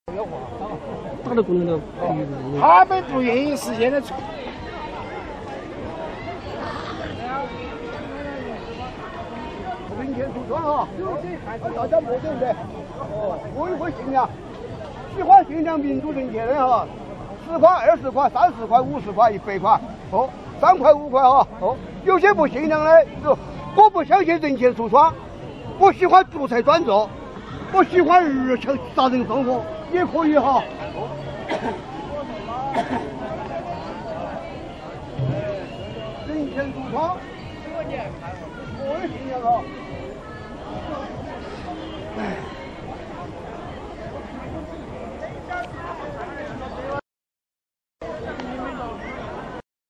贵州民主人士在贵阳市内公园宣讲民主自由
贵州民主人士在贵阳市内公园宣讲民主自由。（贵州人权研讨会提供/记者乔龙） 订阅自由亚洲电台YouTube频道

听众谈话

听众谈话